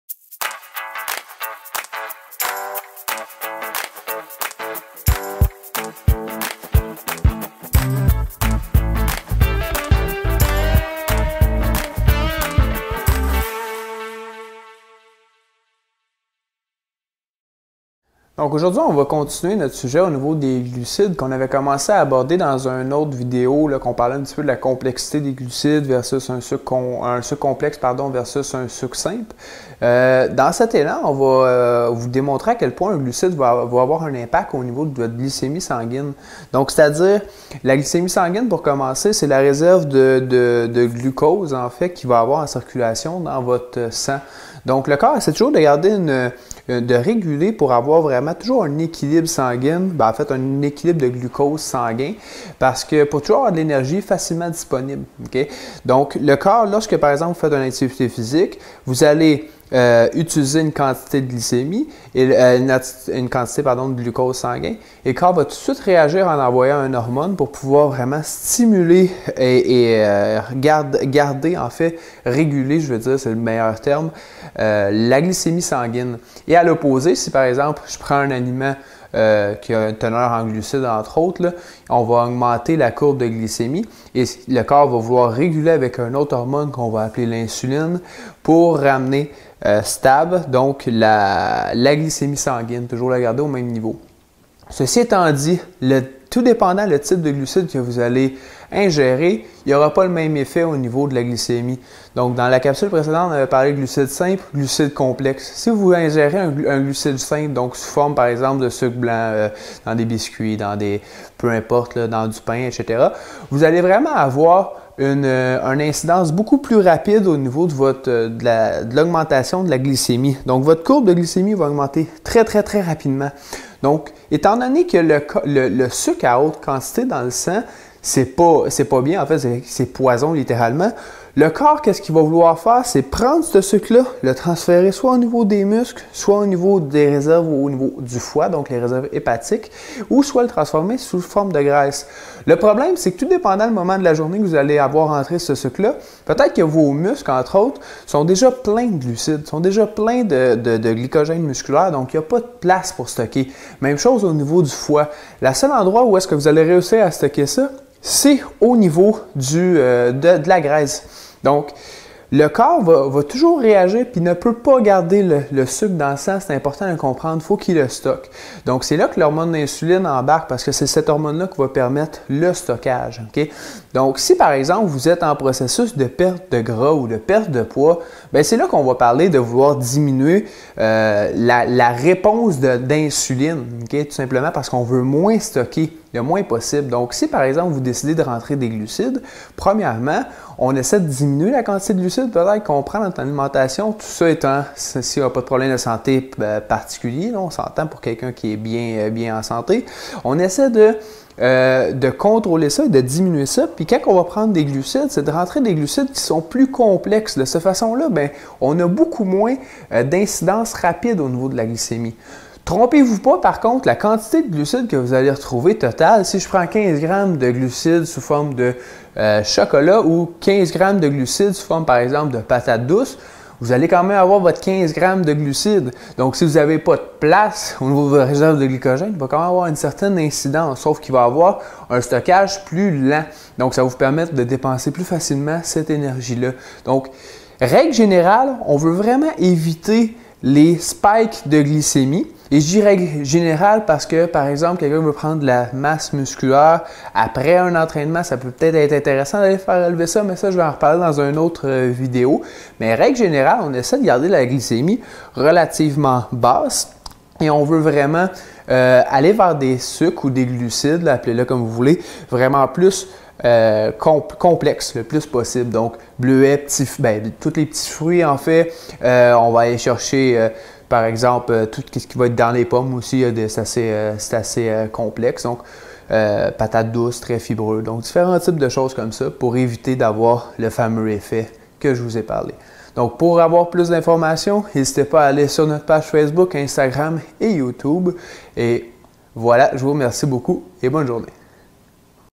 La glycémie sanguine HD 720p
L'impact des glucides sur la glycémie sanguine et la création de tissus adiepux